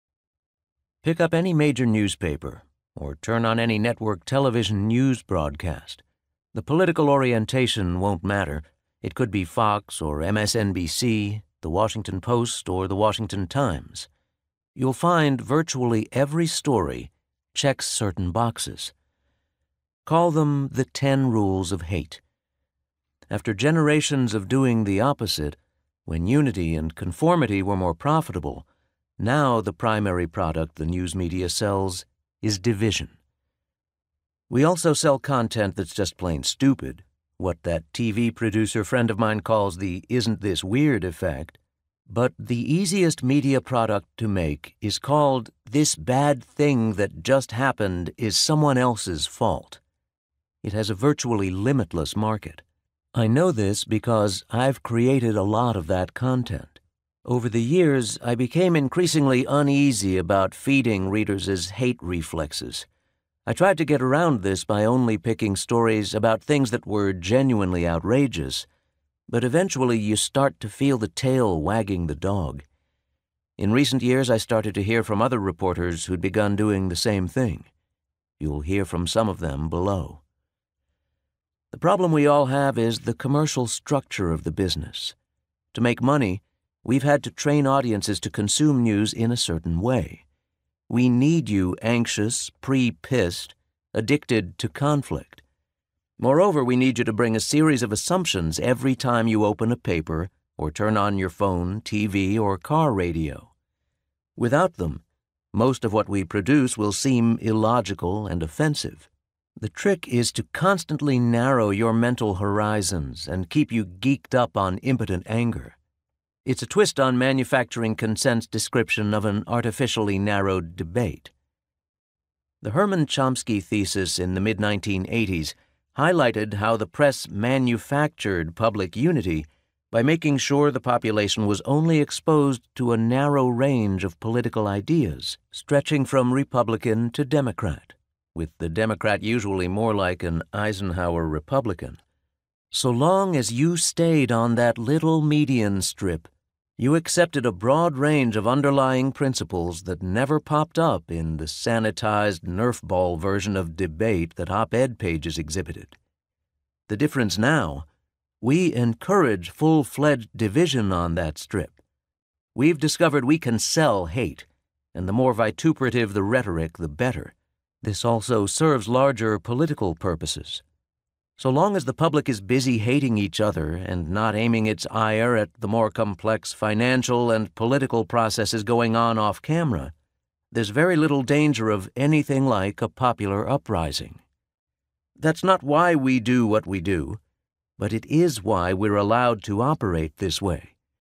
Audiobook Sample: Hate Inc.
Part tirade, part confessional from the celebrated Rolling Stone journalist, Hate Inc. reveals that what most people think of as "the news" is, in fact, a twisted wing of the entertainment business.

In this characteristically turbocharged new book, celebrated Rolling Stone journalist Matt Taibbi provides an insider's guide to the variety of ways today's mainstream media tells us lies. Part tirade, part confessional, it reveals that what most people think of as "the news" is, in fact, a twisted wing of the entertainment business.

In the Internet age, the press have mastered the art of monetizing anger, paranoia, and distrust. Taibbi, who has spent much of his career covering elections in which this kind of manipulative activity is most egregious, provides a rich taxonomic survey of American political journalism's dirty tricks.

Heading into a 2020 election season that promises to be a Great Giza Pyramid Complex of invective and digital ugliness, Hate Inc. will be an invaluable antidote to the hidden poisons dished up by those we rely on to tell us what is happening in the world.